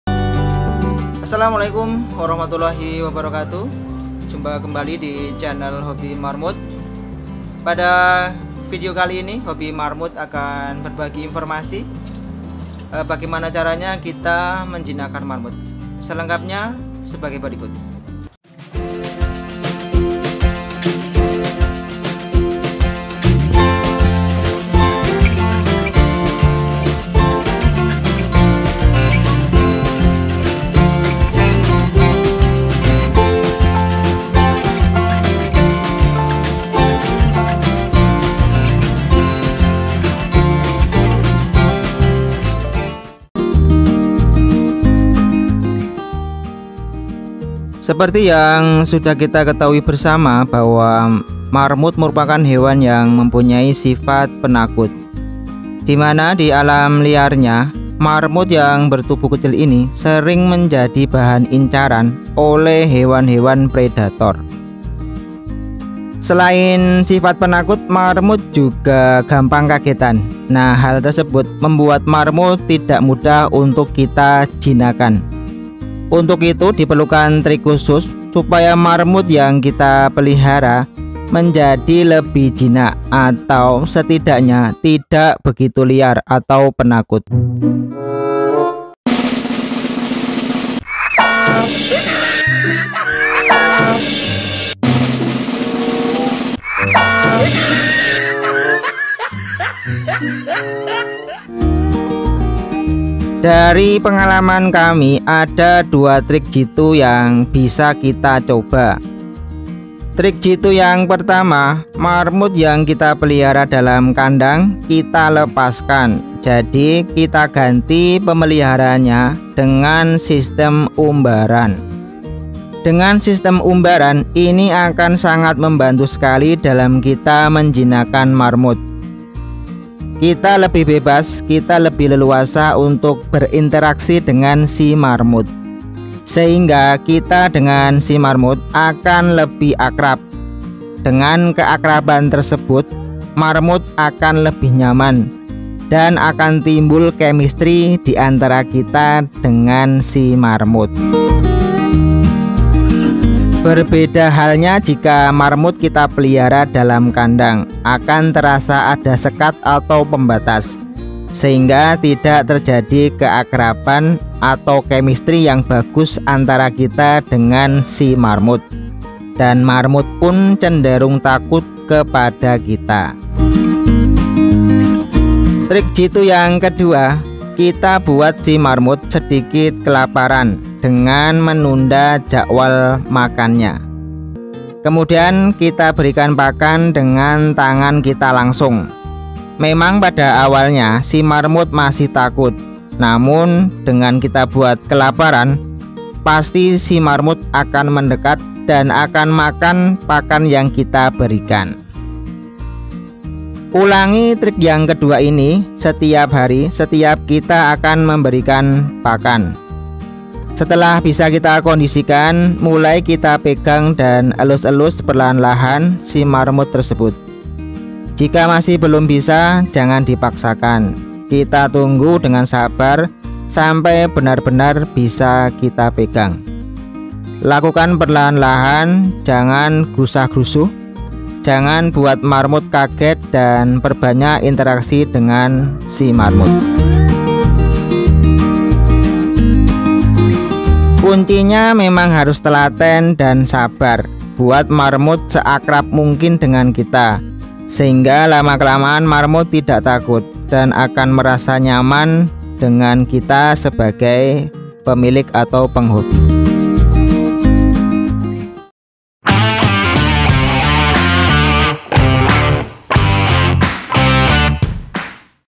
CARA MENJINAKAN MARMUT I Trik Jitu 100% Terbukti InsyaaAllah.
Cara menjinakan marmut tidak terlalu sulit jika paham caranya. Menurut pengalaman kami ada 2 Trik Jitu menjinakan marmut yang bisa dicoba yaitu :
1.Marmut kita pelihara dengan sistem umbaran.
2.Marmut kita berikan makanan dengan tangan kita langsung.
Ulangi trik tersebut sampai benar marmut menjadi jinak, kuncinya harus sabar dan telaten.
Selamat Mencoba dan sukses bagi semua penghobi marmut diseluruh indonesia.